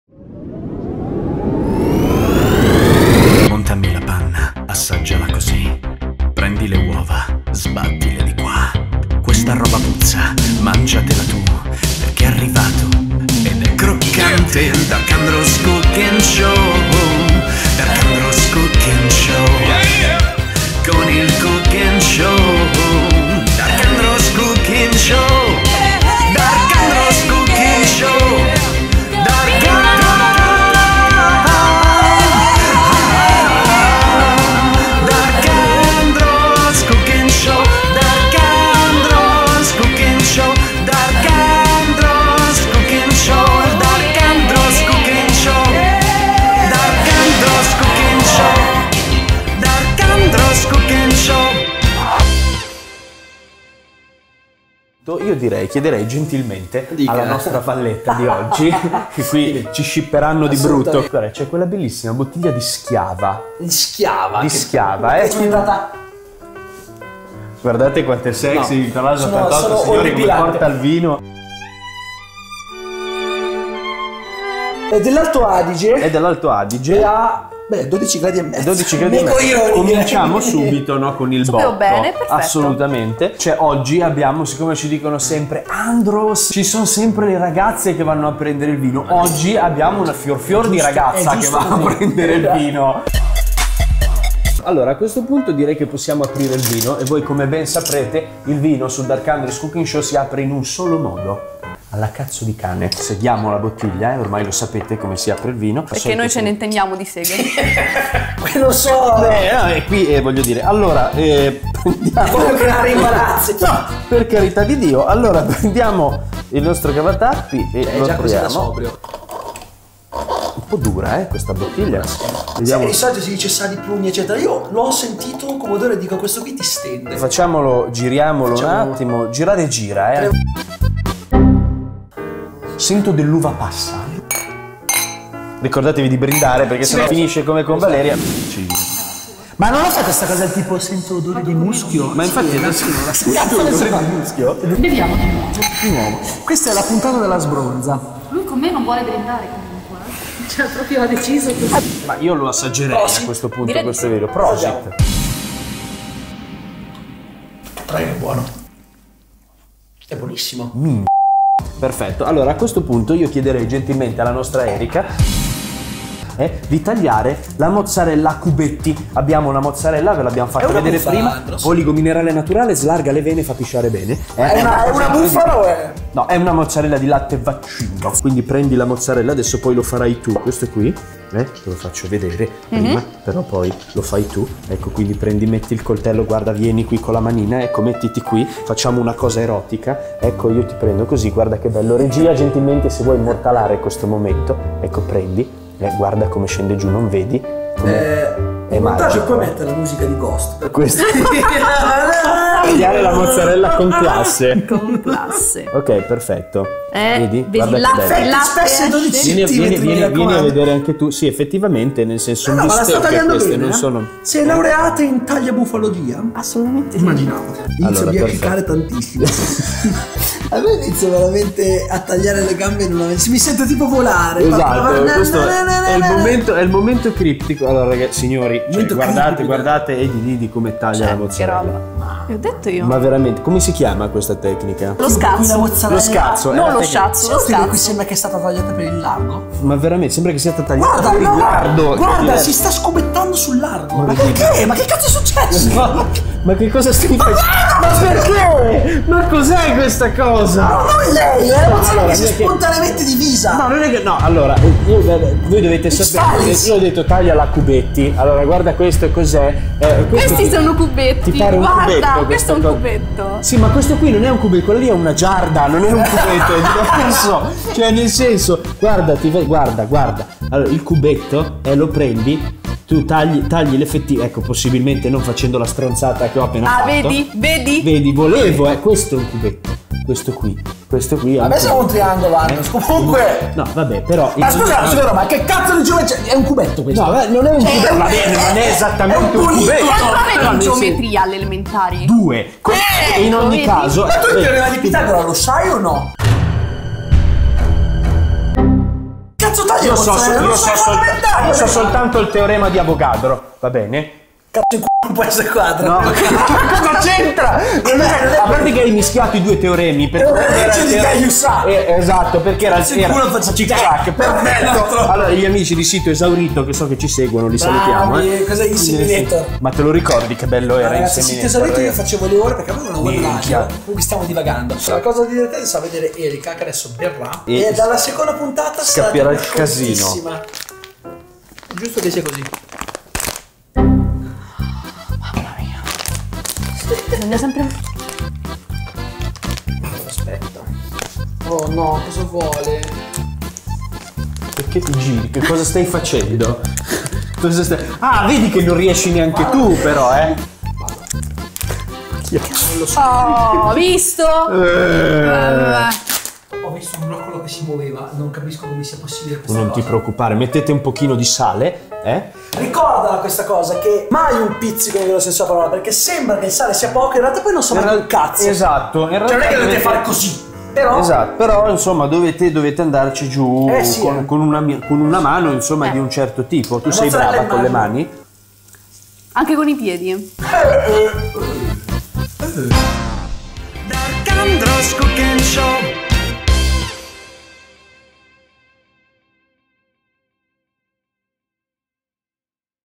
VINOH vol.2 - Lo Schiava con mozzarella - DarkAndross Cooking Show EXTRA
VINOH! vol.2 e come degustarlo in modo croccante :D
Il vino scorre potente in noi, ma Erika sbaglia a tagliare la mozzarella e Victorlaszlo88 si incazza.